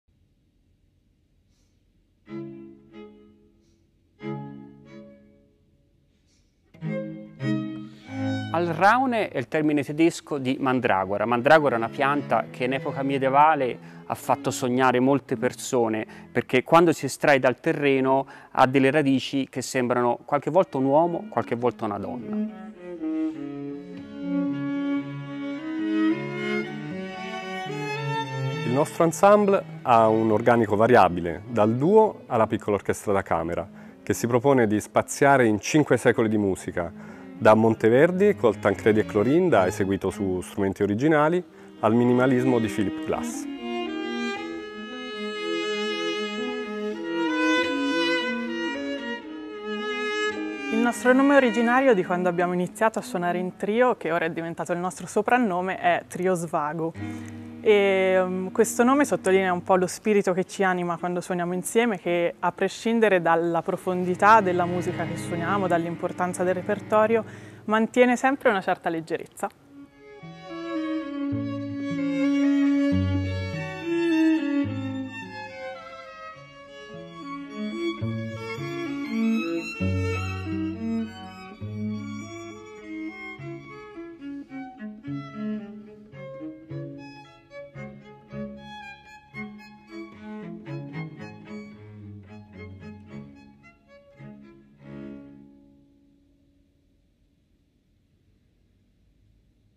Ensemble Alraune
Alraune è il nome tedesco della mandragora (conosciuta anche come mandragola), una pianta della famiglia delle Solanaceae nota fin da tempi antichi sulla quale si è sbizzarrita da sempre la fantasia popolare creando intorno a lei un’aura di mistero. Per noi è essenzialmente un gruppo da camera di dimensione variabile che ama suonare lungo cinque secoli di storia, da Claudio Monteverdi a Philip Glass. Il "gruppo svago" è qui composto da Stefano Zanobini (viola) Augusto Gasbarri (violoncello) e Clara Franziska Schötensack (violino).